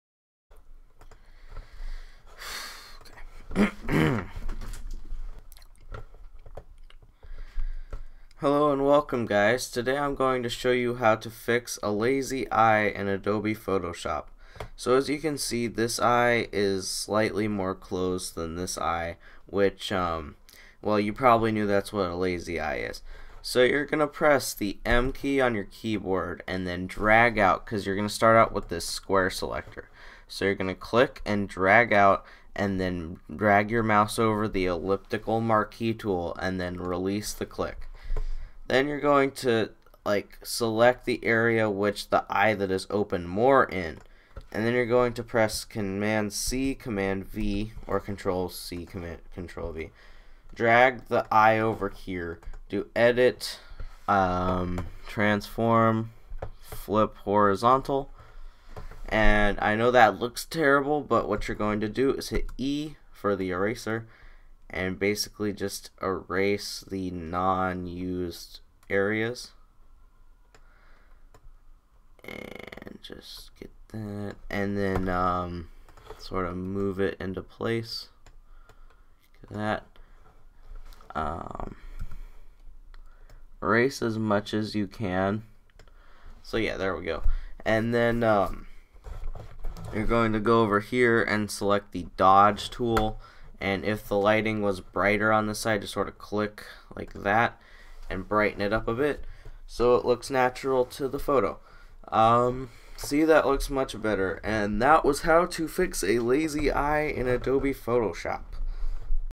How to Fix a Lazy Eye With Adobe Photoshop
This tutorial shows how to fix a lazy eye with simple tools in Adobe Photoshop CS6.